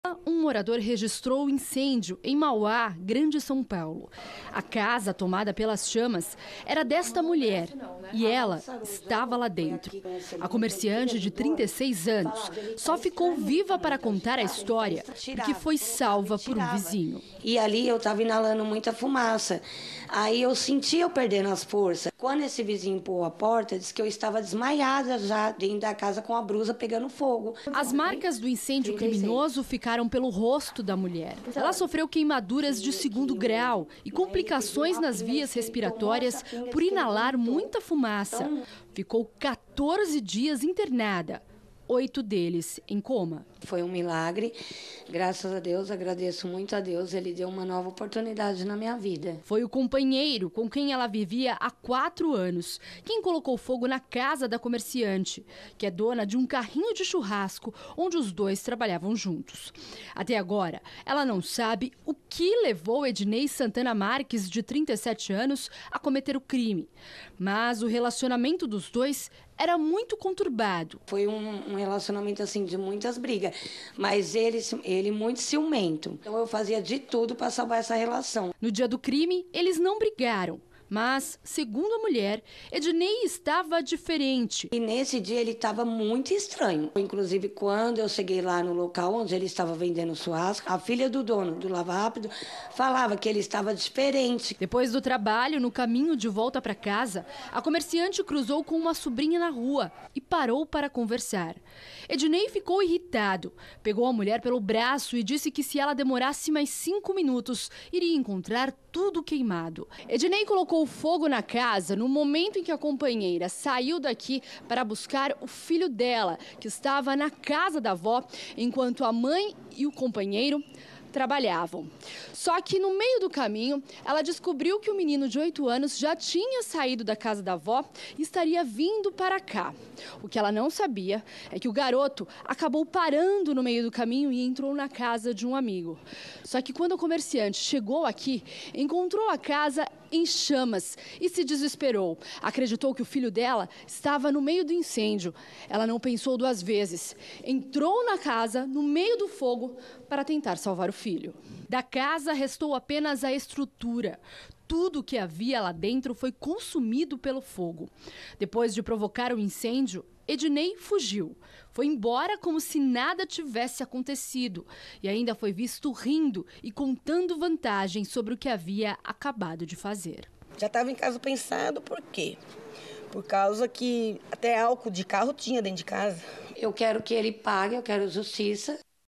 Homem põe fogo na casa da esposa na Grande São Paulo
A comerciante de 36 anos foi salva por um vizinho após ter a casa queimada pelo próprio companheiro. O caso aconteceu em Mauá, na Grande São Paulo.